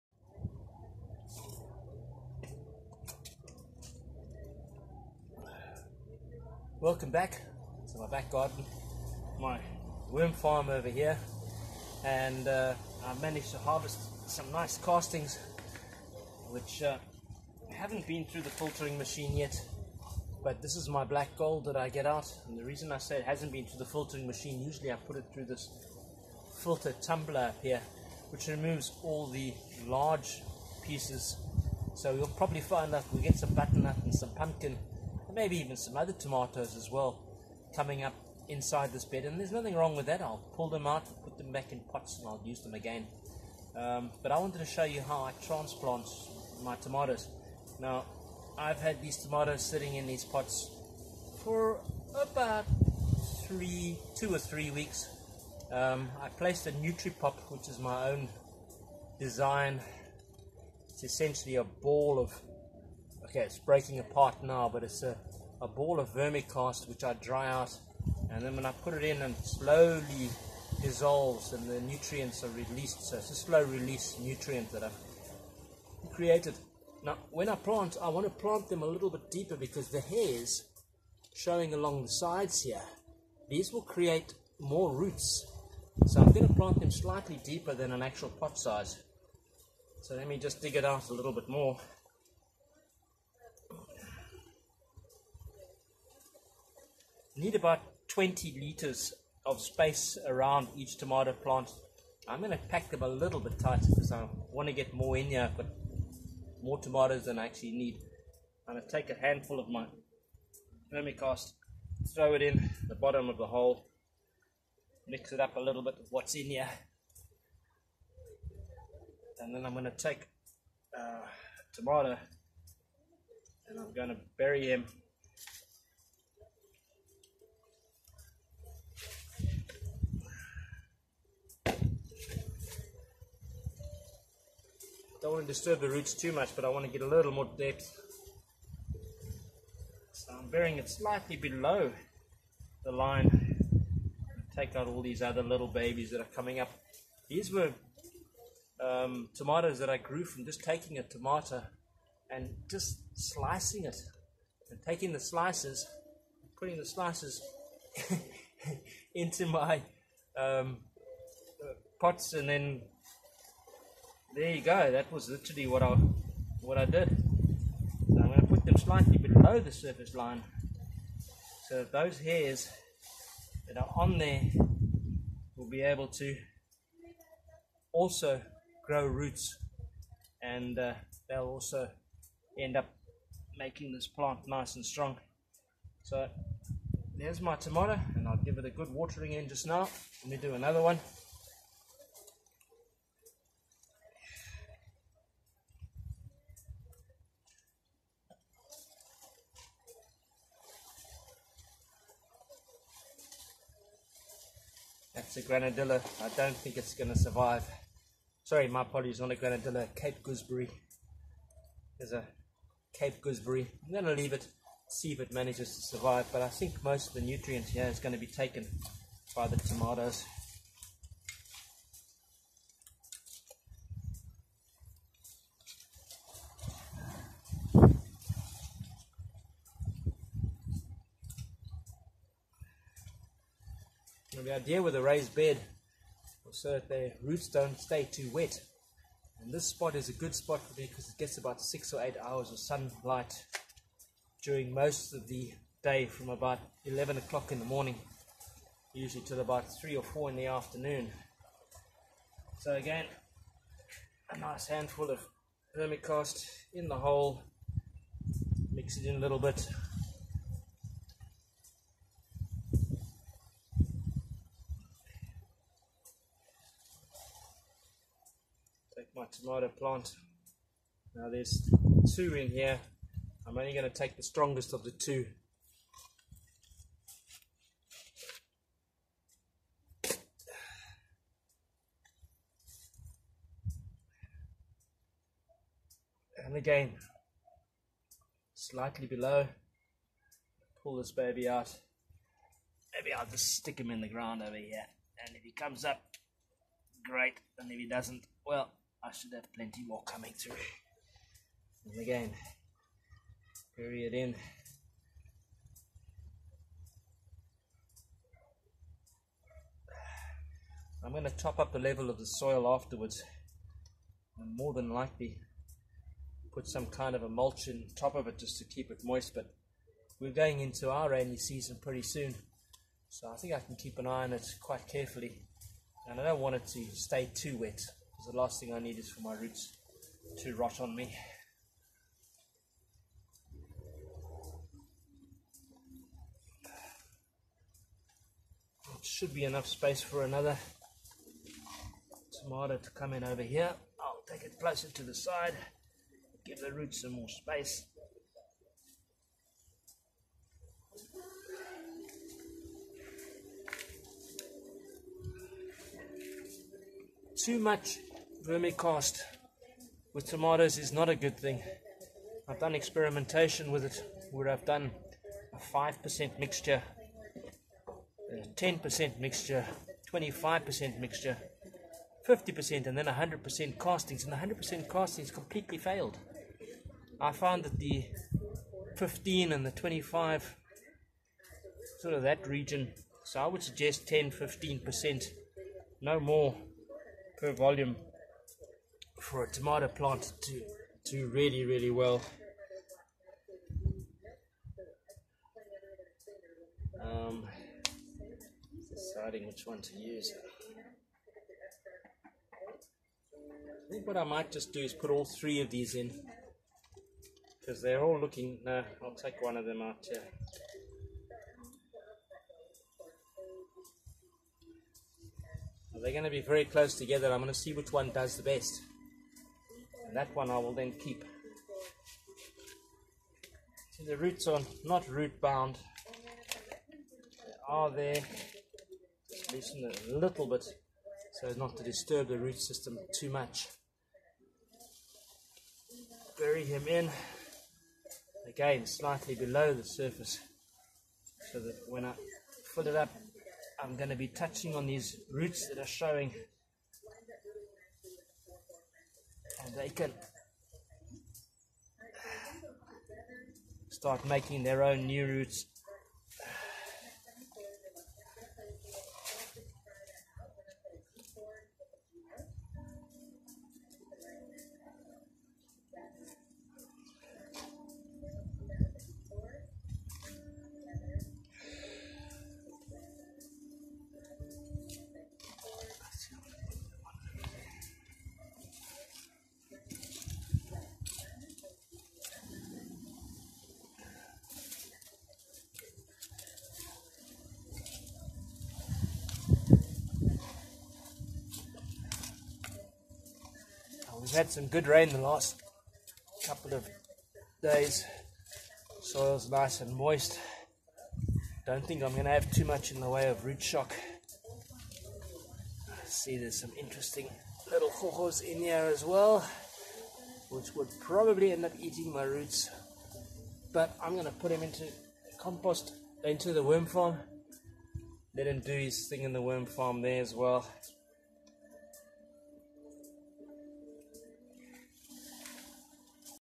How and why to plant tomatoes in a raised bed and planted with vermicast at the root level.
Here I will show you approximately how much space is required, and how much vermicast to use, when planting out your tomato plants. I also throw out a peppering of castings monthly to keep the nutrients going throughout the growing season. It is not advised to wet the leaves when watering or allow the roots to stay wet. Well draining soil is always good and a mulch helps with soil born diseases getting onto the leaves when it rains.